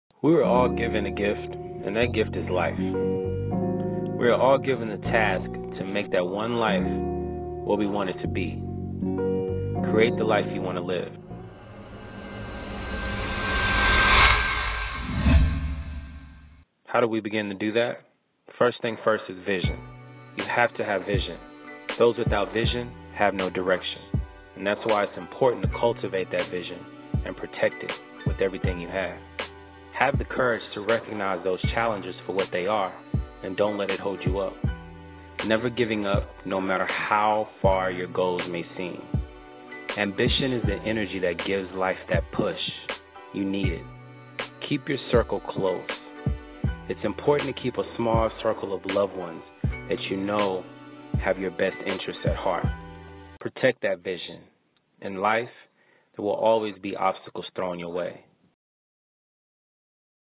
Prince Williams-Reflections on Life
Maryland DUI and criminal defense attorney shares his thoughts on navigating life.